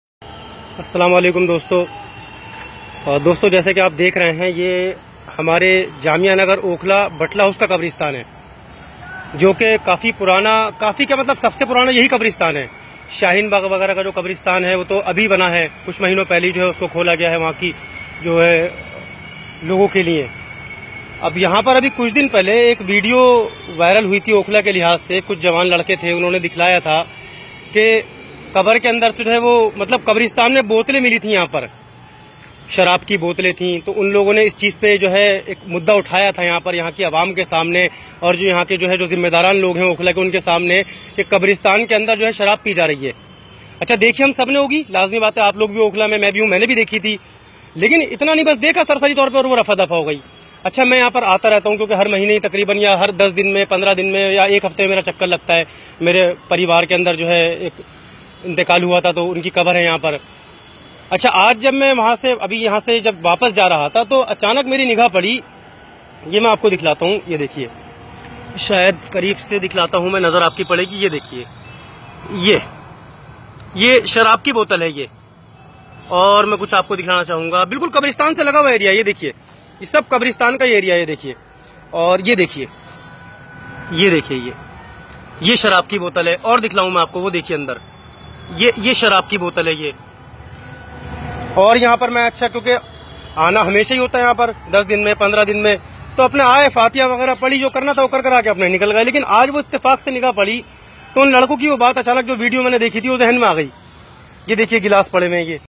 Batla House graveyard
Video of this Batla House graveyard has gone literally viral with residents expressing concern over the content of the video.